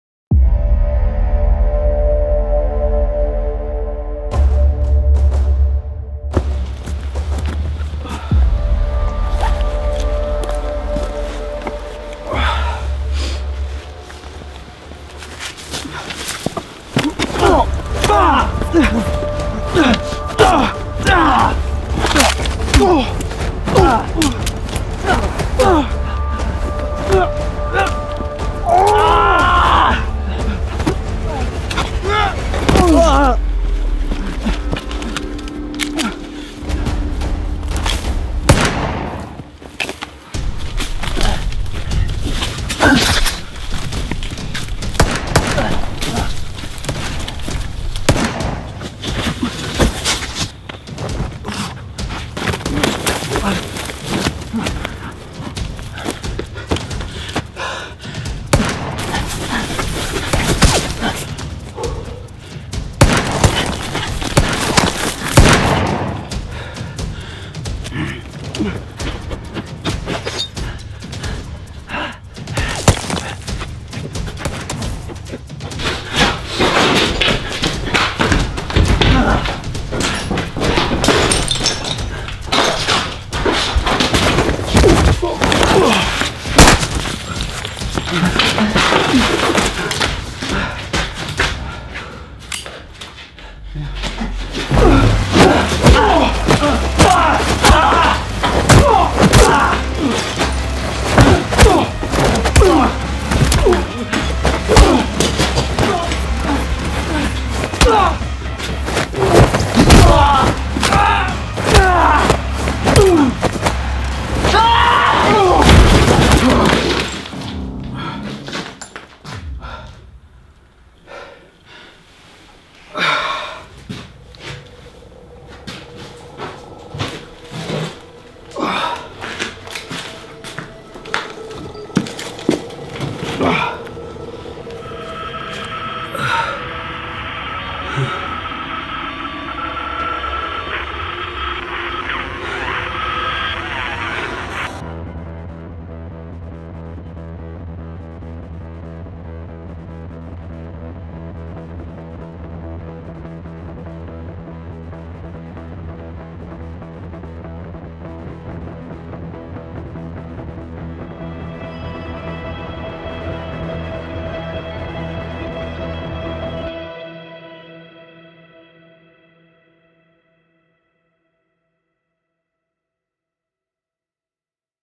STOMP
Après une année rude pour la création de contenu, voici une nouvelle pastille d'action de 2m30, "STOMP"

4h de répétitions et de préparations
6h de tournages
10h environ de post production

After a rough year for content creation, here's a new 2m30 action short film, "STOMP"

4h of rehearsals and preparation
6h of filming
10h of post production